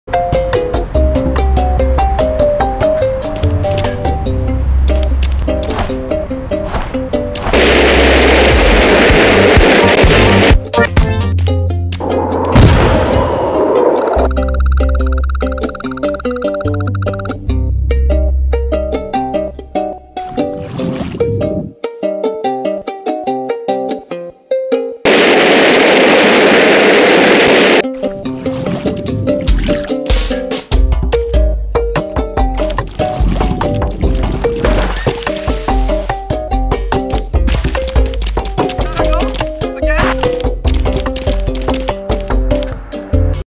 Moon Goat
Our goat here is making solid progress in his astronaut training. His goal is to be the first goat in space.